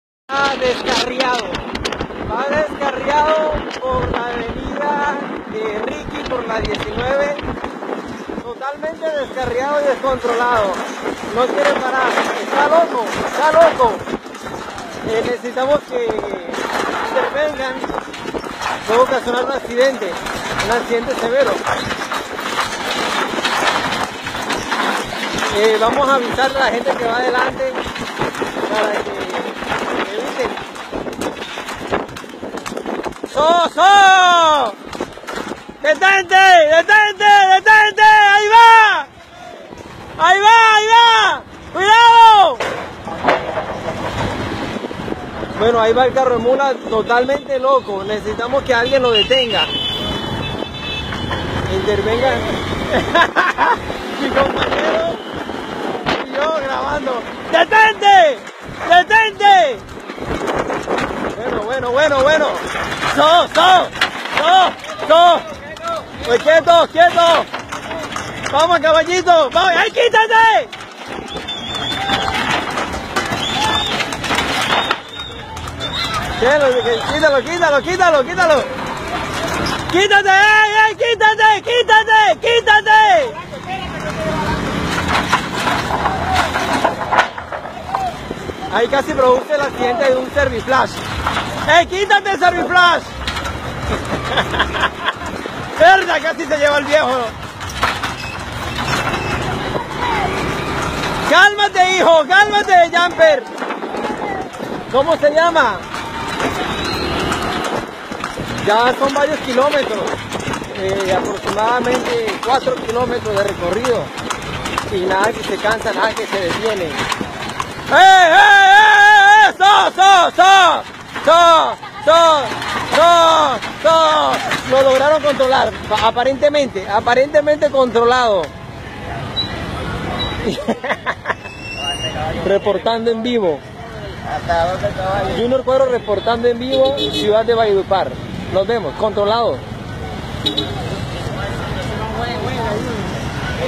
ESTE CABALLO SE VOLVIO LOCO Y CORRIO VARIOS KILOMETROS HASTA SER ALCANZADO
Un peligro.